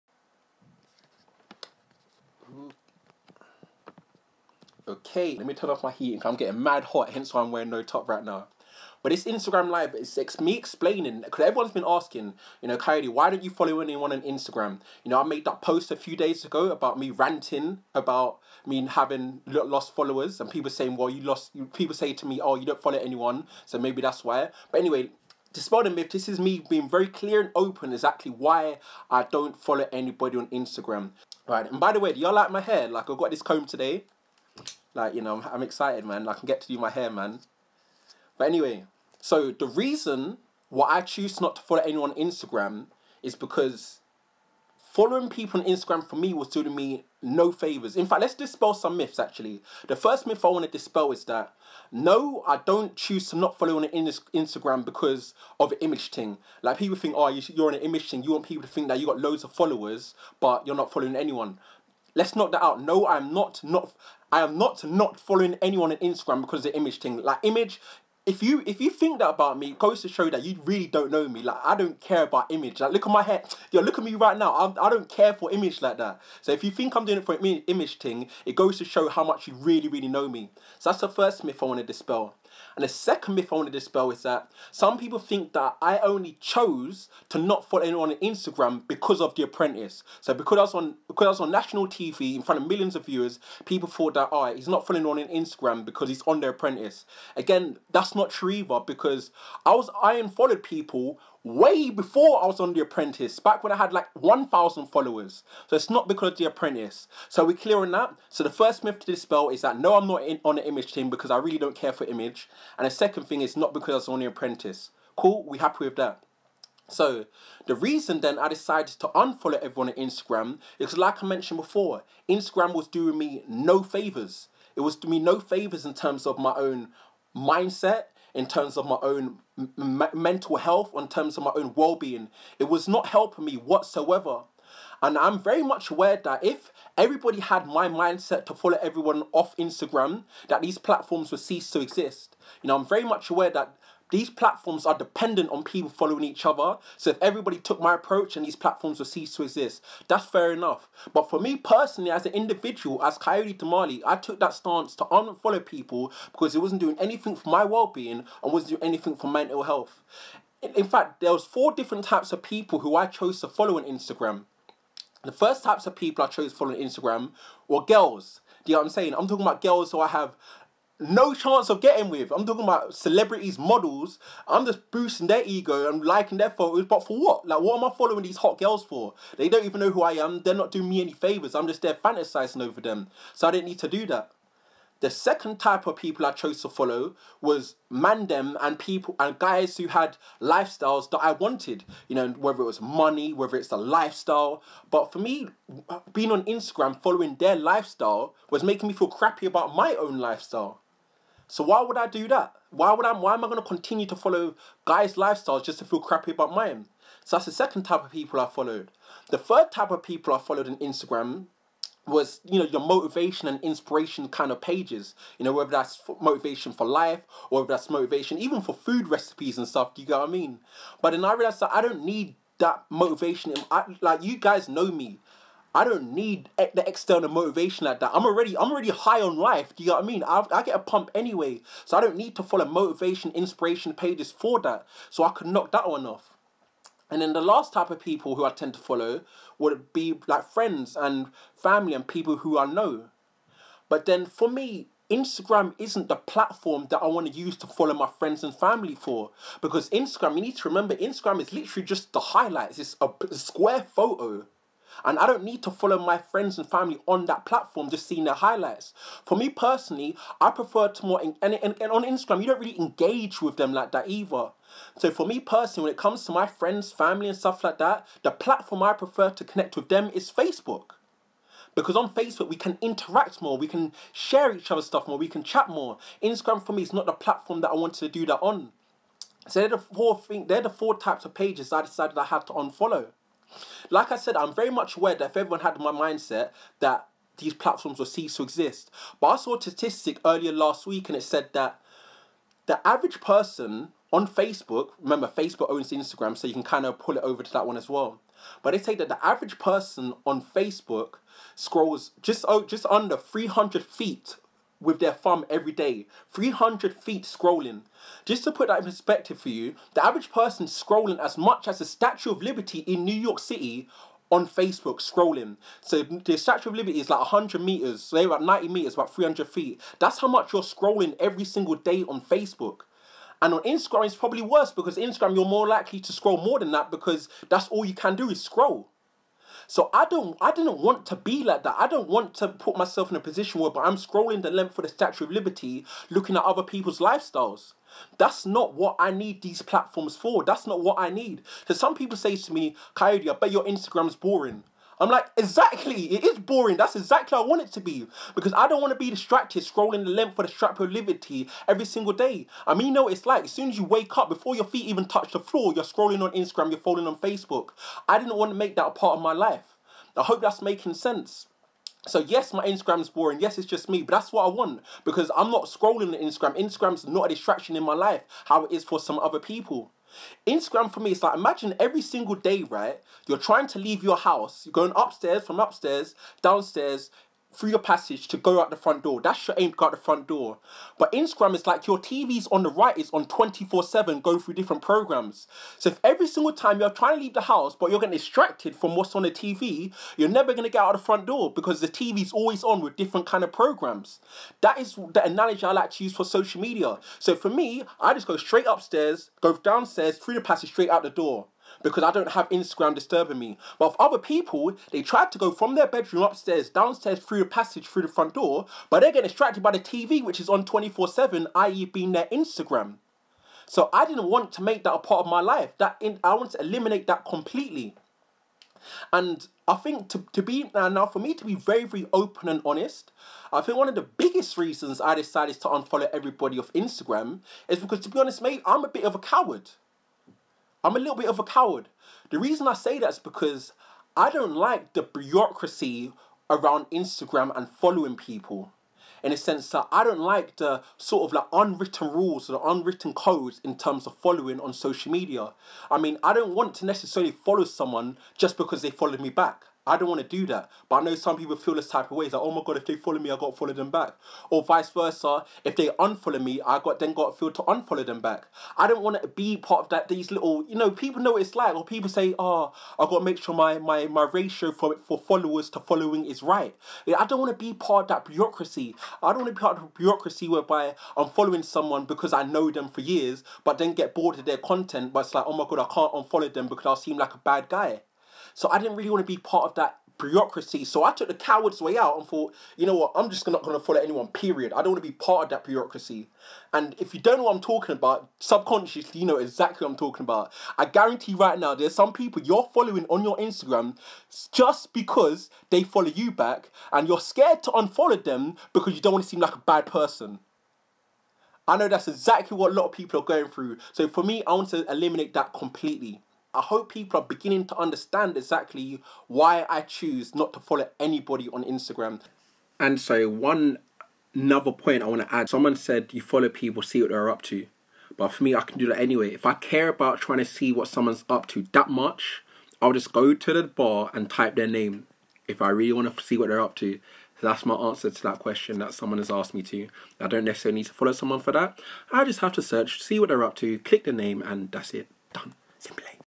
Why I don't follow anyone on Instagram and it's not why you think...
Video taken on Instagram Live explaining the REAL TRUTH why I don't follow anyone and eradicating the myths...